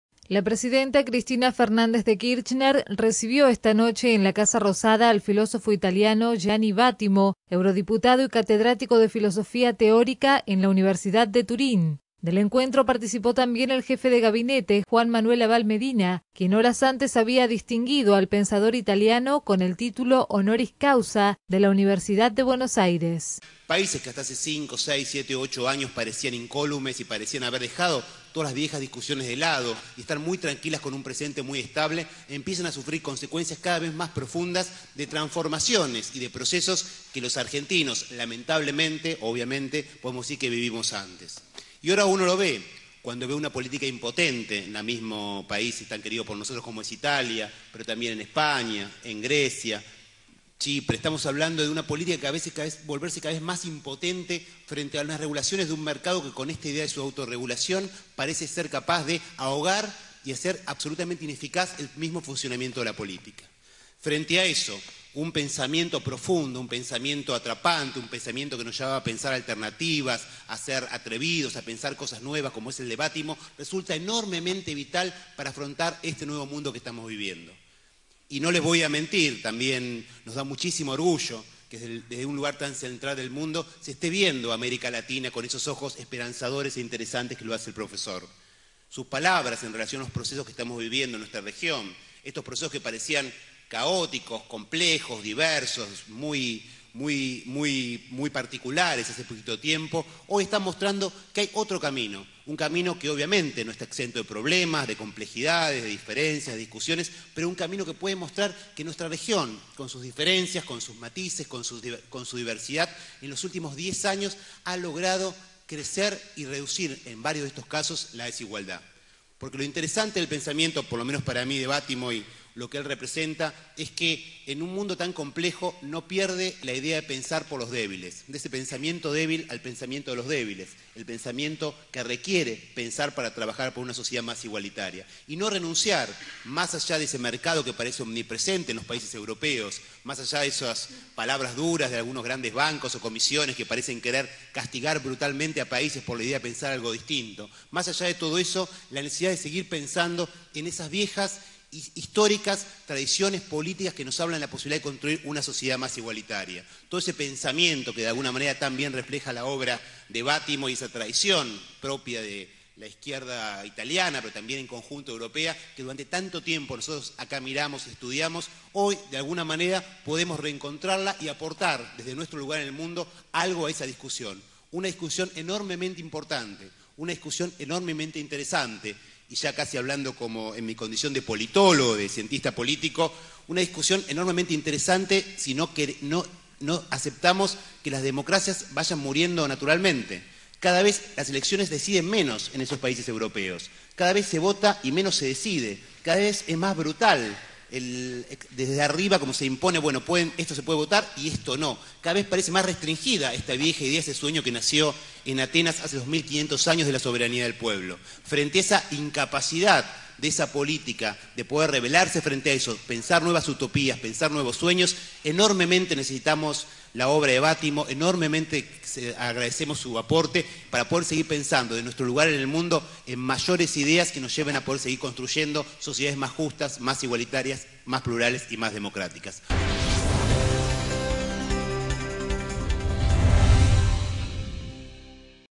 La Presidenta recibió al filósofo Gianni Vattimo.
La Presidenta Cristina Fernández de Kirchner recibió esta noche en la Casa Rosada al filósofo italiano Gianni Vattimo, eurodiputado y catedrático de Filosofía Teorética en la Universidad de Turín.
Del encuentro participó también el jefe de Gabinete, Juan Manuel Abal Medina, quien horas antes había distinguido al pensador italiano con el título Honoris Causa de la Universidad de Buenos Aires (UBA).

Durante la ceremonia de entrega de la distinción, en la que estuvo presente el rector Rubén Hallú, entre otras autoridades y trabajadores de esa casa de estudios, el jefe de Gabinete agradeció haber sido convocado para reconocer a "un pensador con la relevancia y la actualidad que tiene Vattimo.

Para Abal Medina, se trata de "uno de los personajes más interesantes a la hora de aportar nuevas categorías para ayudarnos a comprender este complejo mundo que estamos viviendo.

En ese marco, el ministro puntualizó que naciones que hasta hace poco parecían estables, empezaron a sufrir las consecuencias más profundas de la crisis económica, como ejemplo, nombró a España, Grecia, Italia y Chipre.

Además, Abal Medina destacó su orgullo por las palabras de Vattimo sobre los procesos que está viviendo América Latina que demuestran que ahora se está viendo a nuestra región con ojos esperanzados.

Aseveró que nuestra región, con sus diferencias y matices, hace 10 años está mostrando que hay otro camino y ha logrado crecer y reducir la desigualdad.